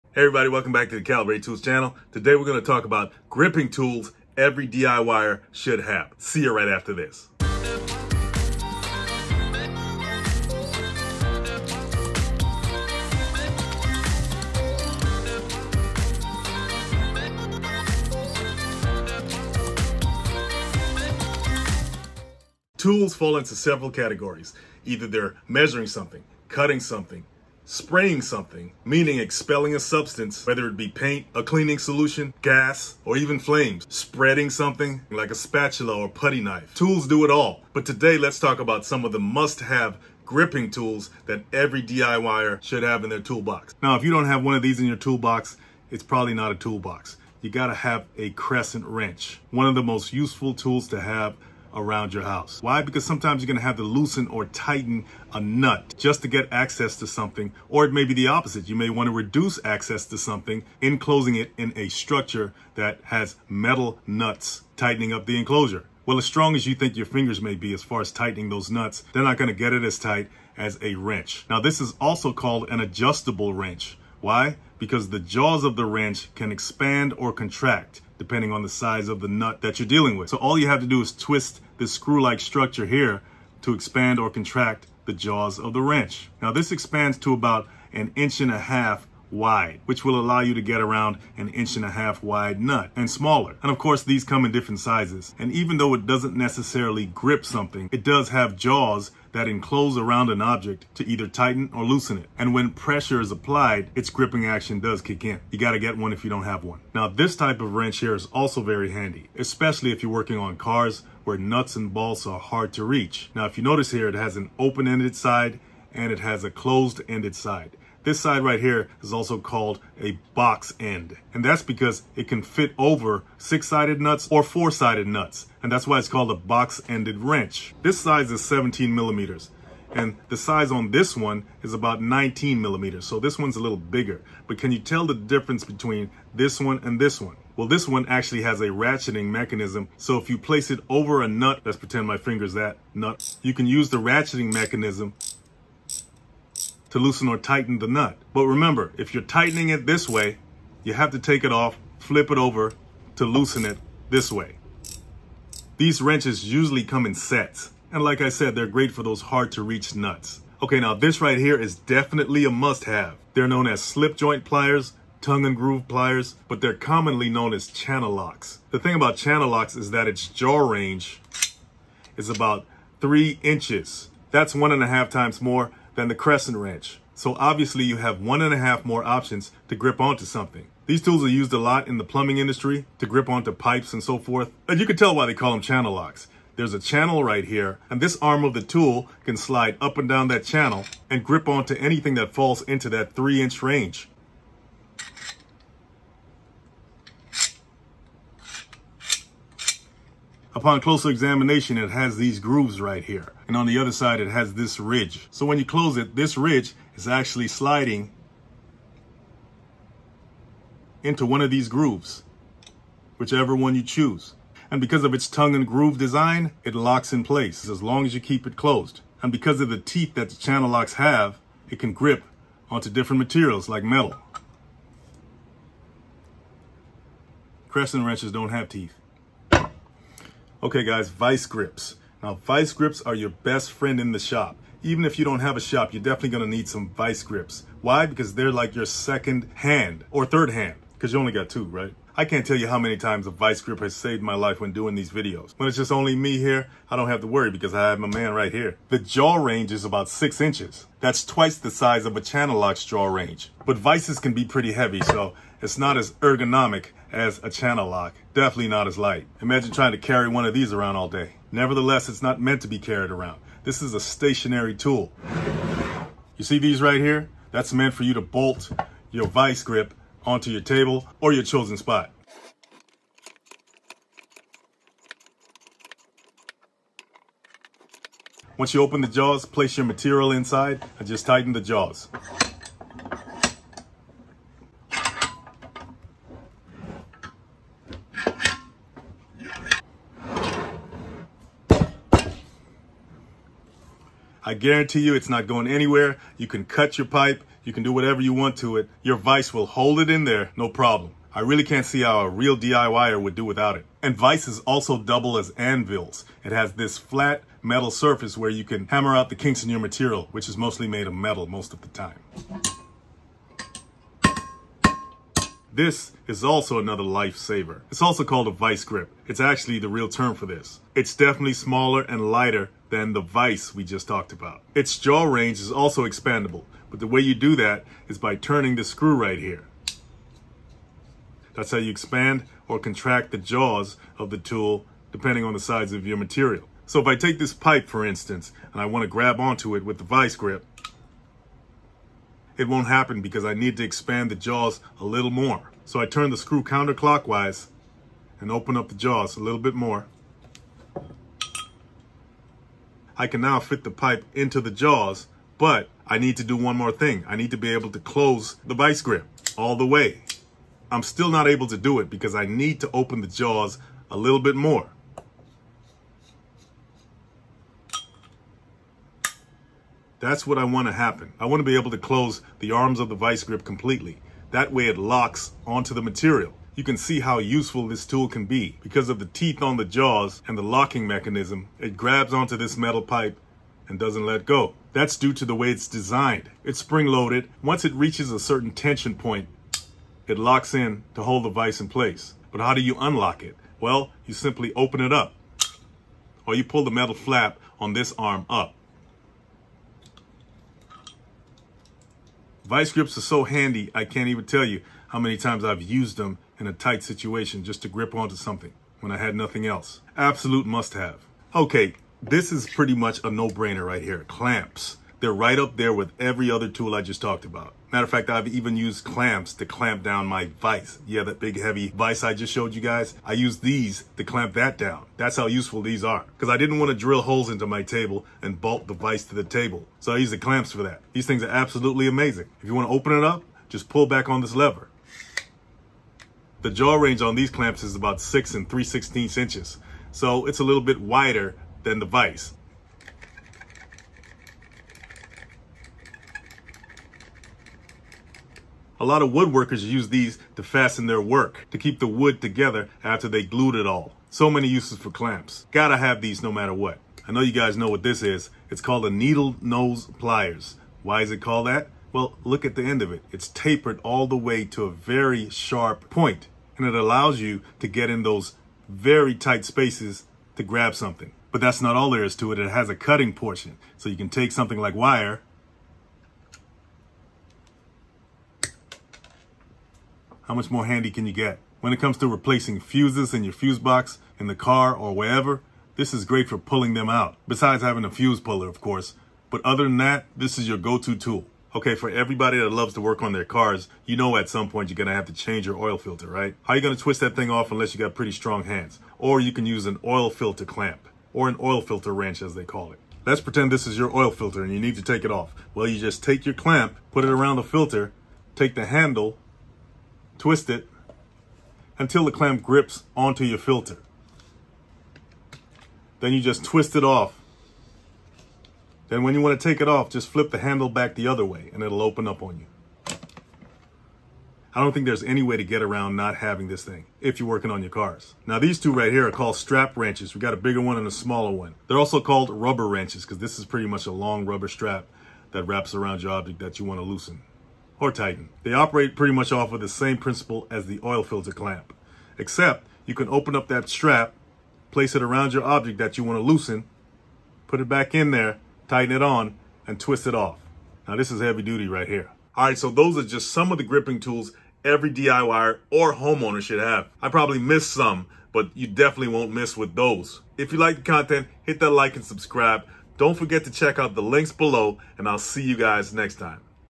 TOOLS THAT MAKE GRIPPING A LOT EASIER
Lets talk about must have gripping tools every DIY'er should have.

Sign up for the email list. Grab some great tools and supplies while supporting the channel @: